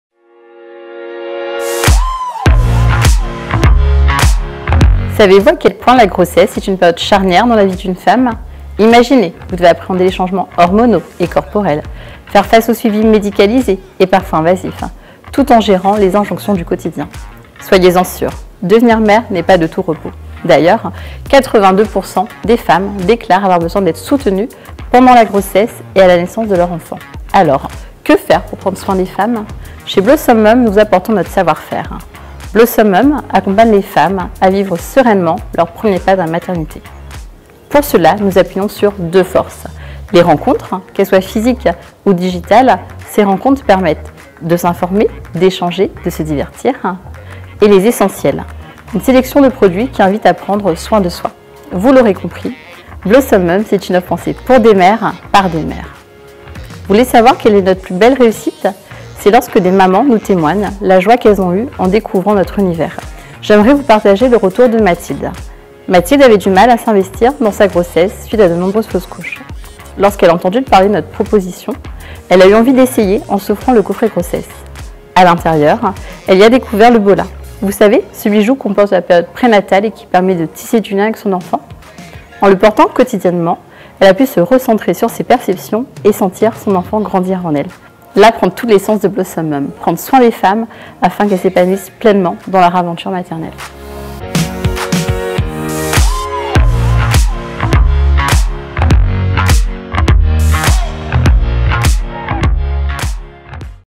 La Cabine • Blossom Mum
Découvrez Blossom Mum, lauréat La Cabine 2020, l'accélérateur de startups de la CCI Vendée.